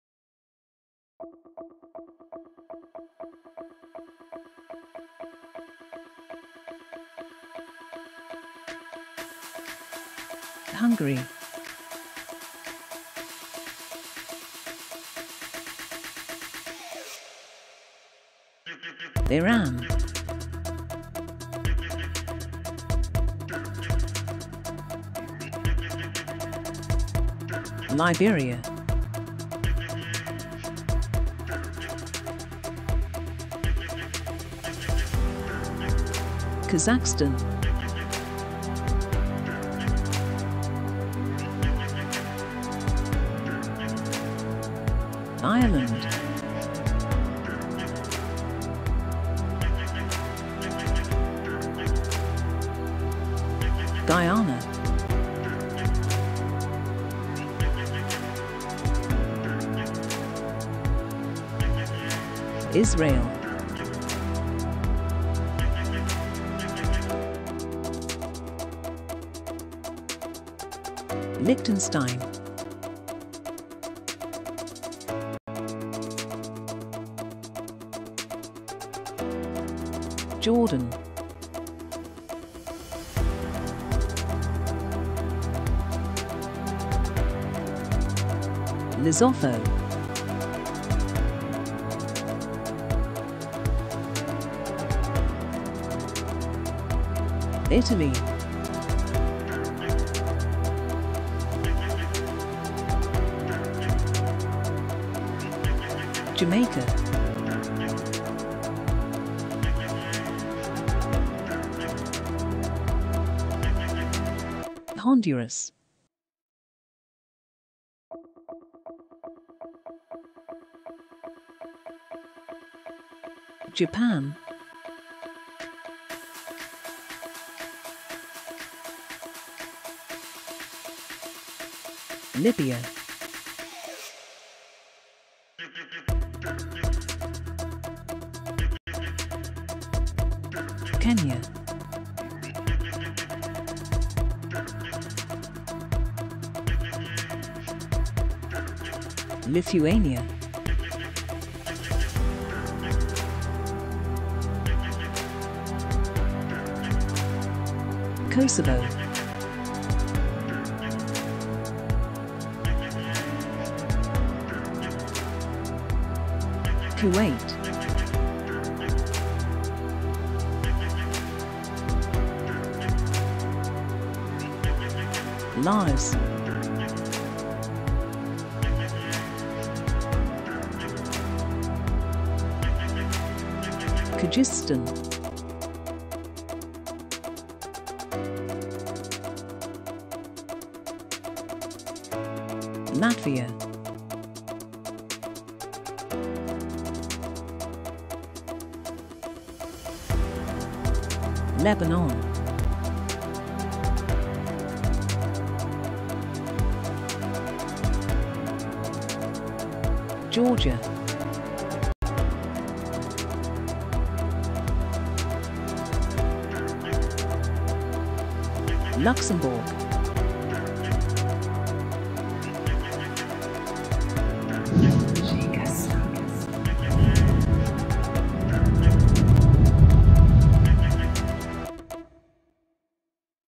Guess the Country Part 4 | 25 Countries | Maps and Flags | Guess in 5 Seconds
Guess the Country in 5 Seconds

Countries included: Hungary, Iran, Liberia, Kazakhstan, Ireland, Guyana, Israel, Liechtenstein, Jordan, Lesotho, Italy, Jamaica, Honduras, Japan, Libya,  Kenya, Lithuania, Kosovo, Kuwait, Laos, Kyrgyzstan, Latvia, Lebanon, Georgia,  Luxembourg